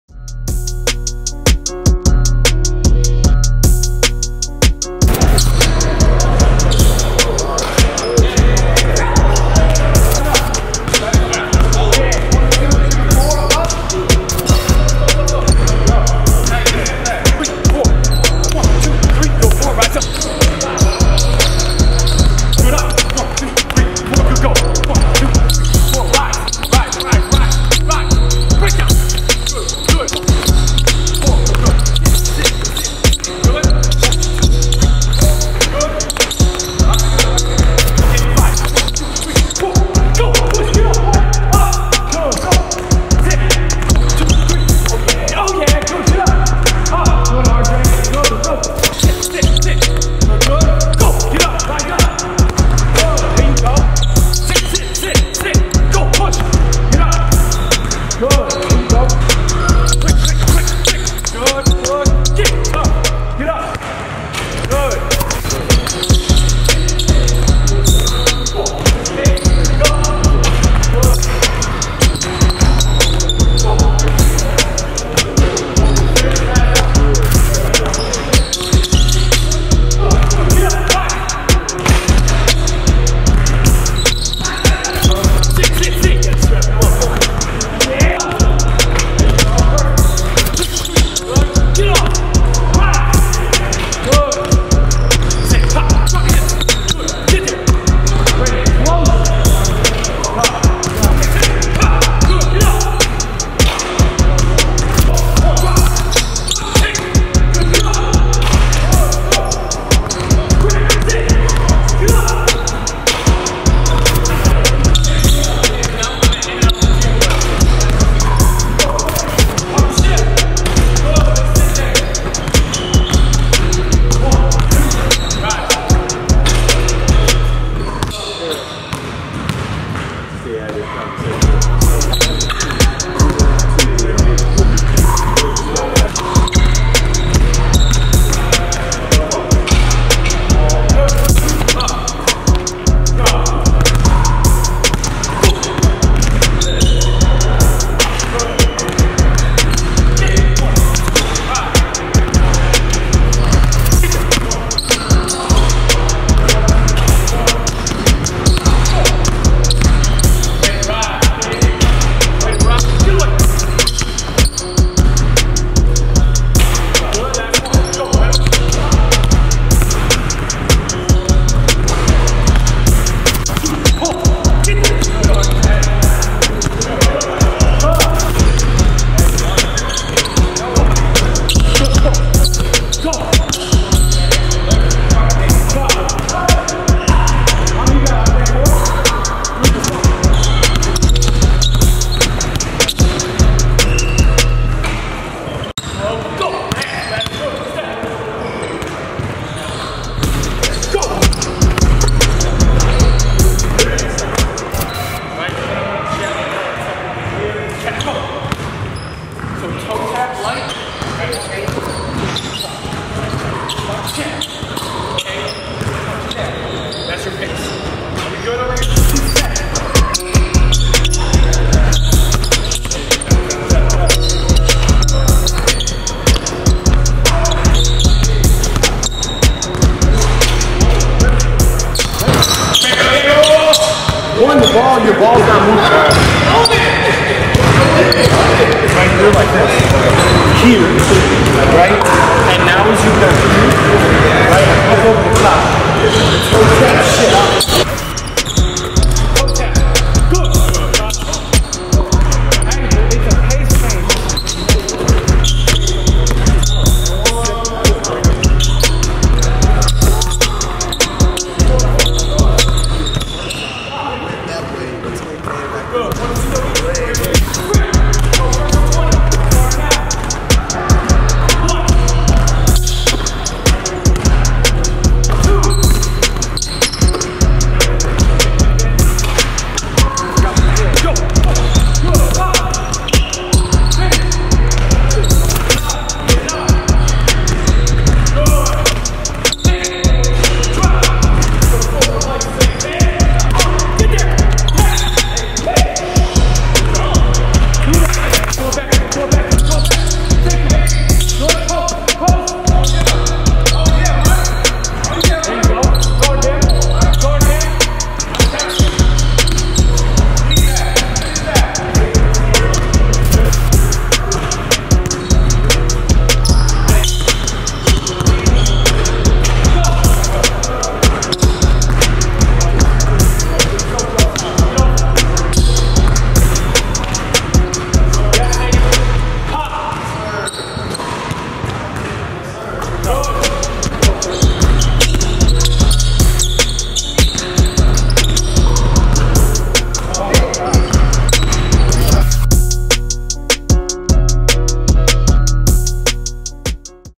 NBA Draft Junkies | Sekou Doumbouya & RJ Hampton workout with Tyler Relph
NBA prospects Sekou Doumbouya and RJ Hampton workout with Tyler Relph on June 1, 2019